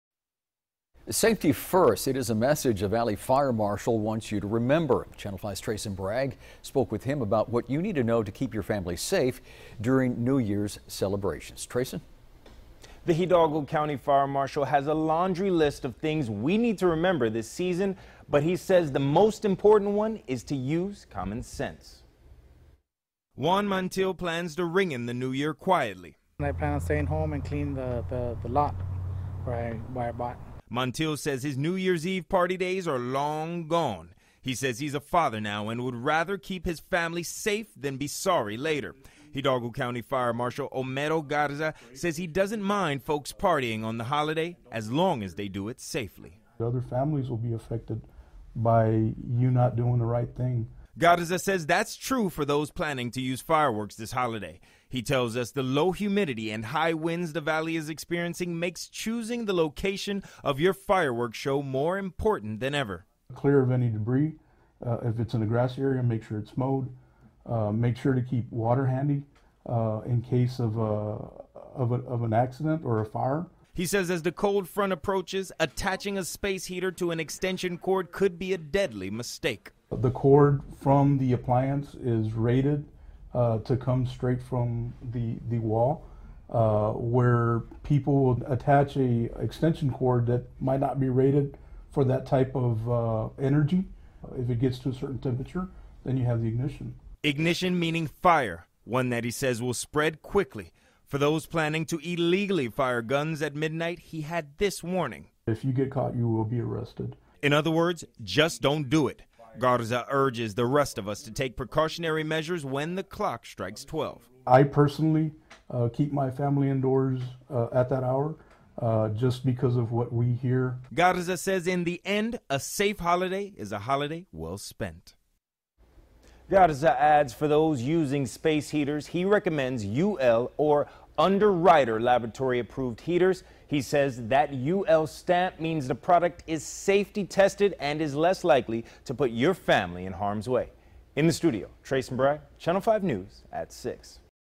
Valley Fire Marshal Urging Public to Stay Safe during New Years Celebrations
He explains the low humidity and high winds the Valley is experiencing makes choosing the location of a fireworks show more important than ever.